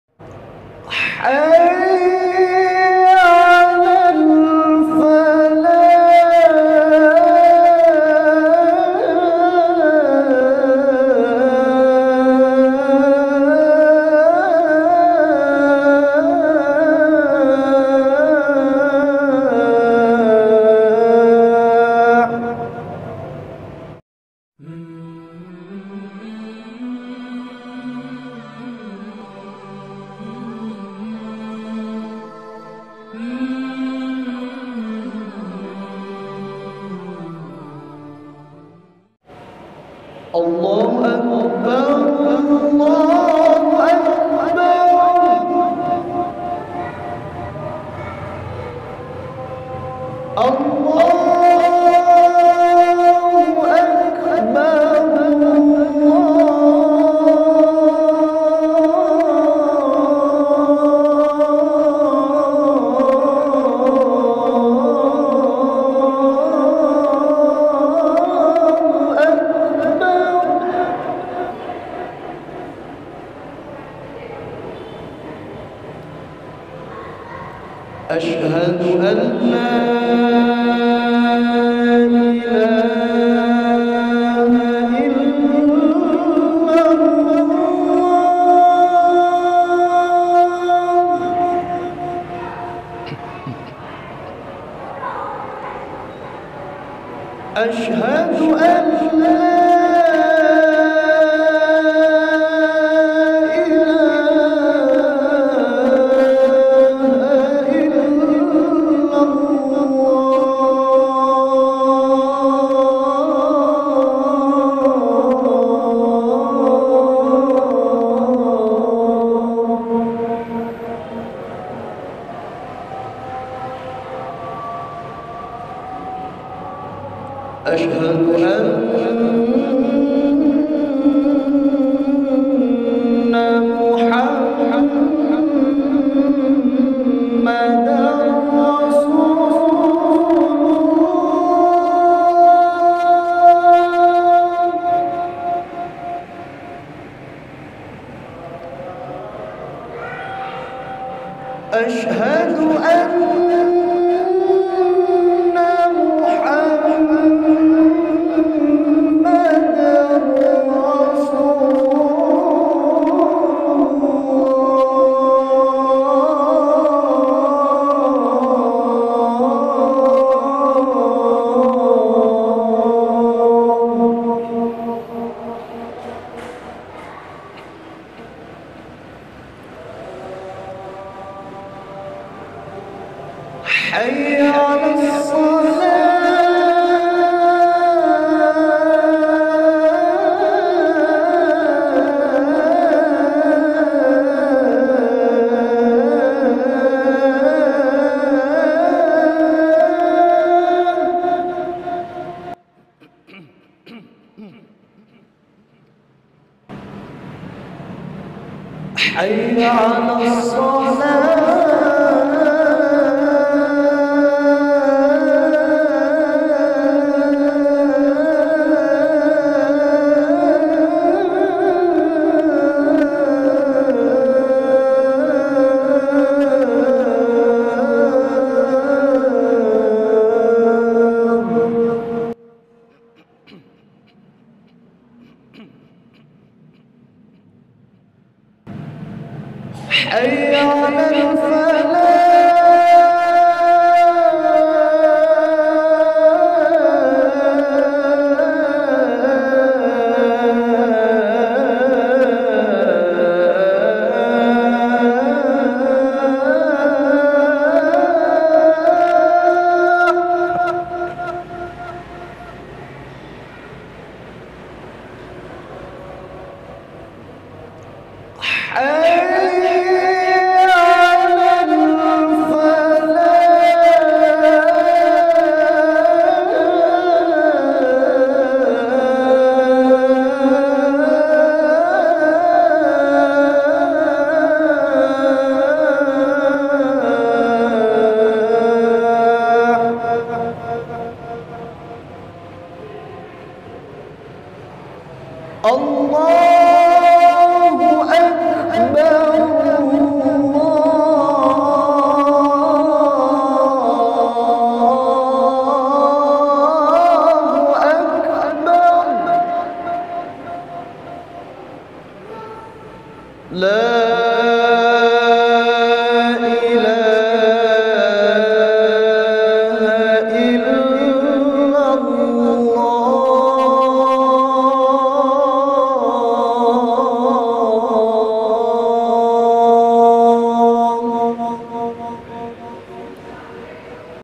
VIRAL...!!! Adzan Maghrib Menjelang BUKA PUASA jdi Ingin Pulang Kampung Sekarang
Sahabat Quran Semoga kalian semua di berikan kemudahan dalam segala Urusannya

di video kali ini sya membuat Video Adzan dengan Judul "VIRAL...!!!  Adzan Maghrib Menjelang BUKA PUASA jdi Ingin Pulang Kampung Sekarang.
mudah"an bisa membuat para pendengar dan Sahabat Quran bisa merasakan kedamaian dalam irama adzan dan lebih menambah serta meningkatkan keimanan dan ketaqwaan kepada Alloh SWT Amiiin ya robbal 'alamin